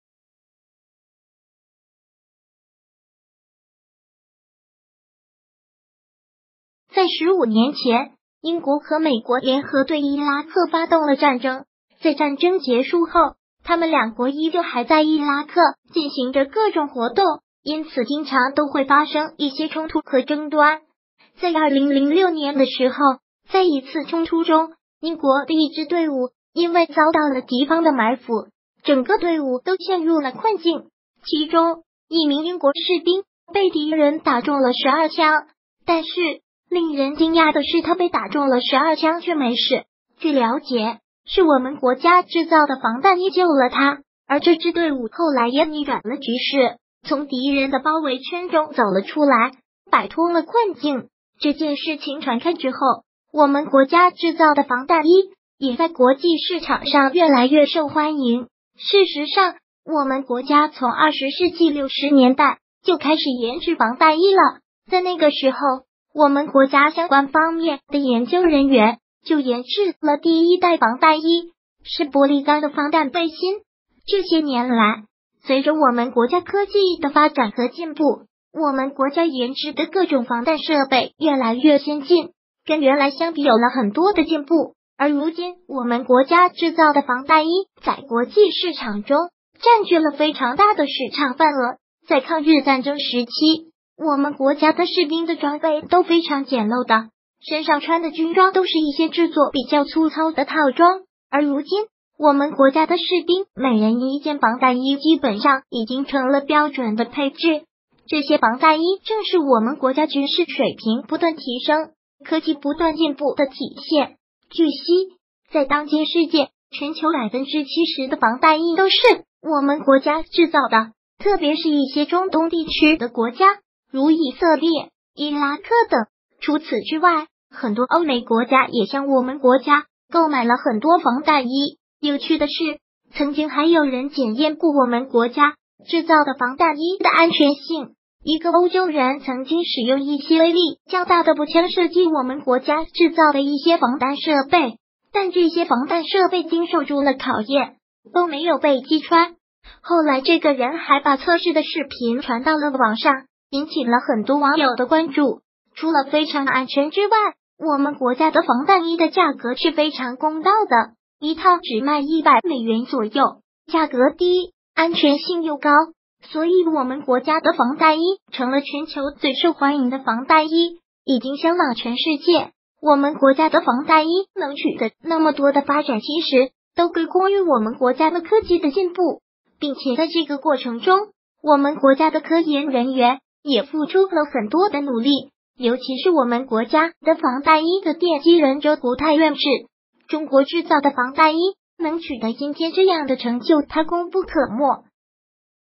中国防弹衣救了英国士兵一命，挨了12枪都没事，随后在黑市火爆
在十五年前，英国和美国联合对伊拉克发动了战争，在战争结束后，他们两国依旧还在伊拉克进行着各种活动，因此经常都会发生一些冲突和争端。在2006年的时候，在一次冲突中，英国的一支队伍因为遭到了敌方的埋伏，整个队伍都陷入了困境，其中，一名英国士兵被敌人打中了12枪，但是，令人惊讶的是他被打中了12枪却没事，据了解，是我们国家制造的防弹衣救了他。而这支队伍后来也逆转了局势，从敌人的包围圈中走了出来，摆脱了困境。这件事情传开之后，我们国家制造的防弹衣也在国际市场上越来越受欢迎。事实上，我们国家从二十世纪六十年代就开始研制防弹衣了，在那个时候，我们国家相关方面的研究人员就研制了第一代防弹衣，是玻璃钢的防弹背心。这些年来，随着我们国家科技的发展和进步，我们国家研制的各种防弹设备越来越先进，跟原来相比有了很多的进步。而如今我们国家制造的防弹衣在国际市场中占据了非常大的市场份额。在抗日战争时期，我们国家的士兵的装备都非常简陋的，身上穿的军装都是一些制作比较粗糙的套装，而如今，我们国家的士兵每人一件防弹衣基本上已经成了标准的配置。这些防弹衣正是我们国家军事水平不断提升，科技不断进步的体现。据悉，在当今世界